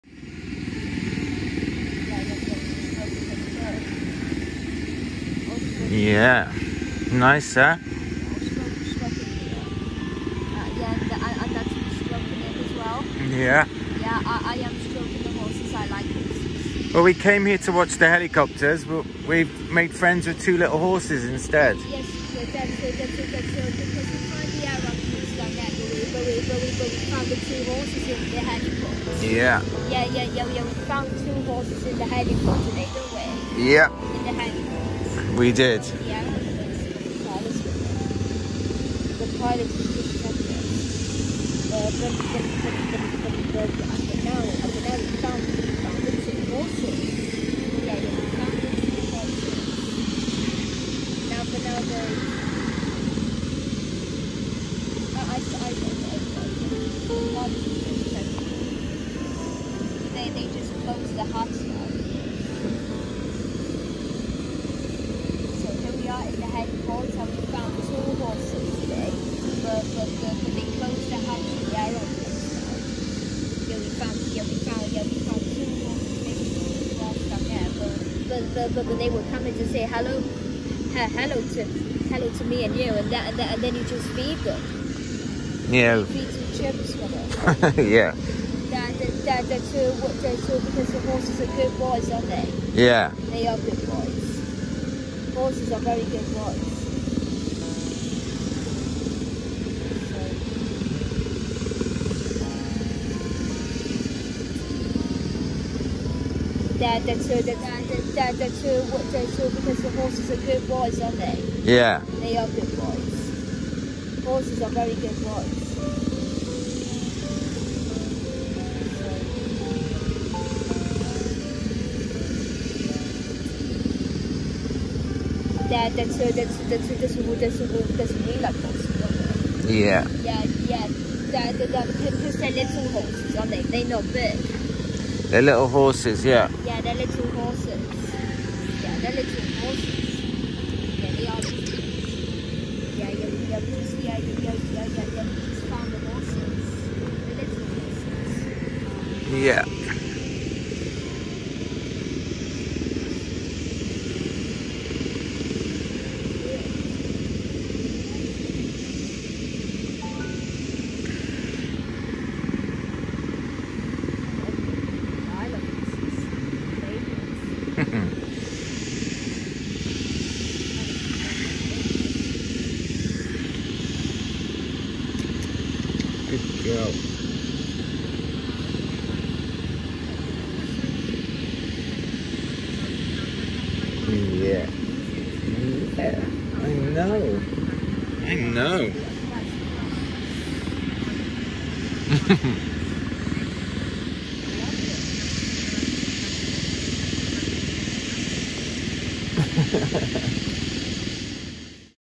Richard and Jaco ‘HeliHorses’
Richard and Jaco go to see the air ambulance land at Cardiff Heliport & make friends with 2 little horses instead!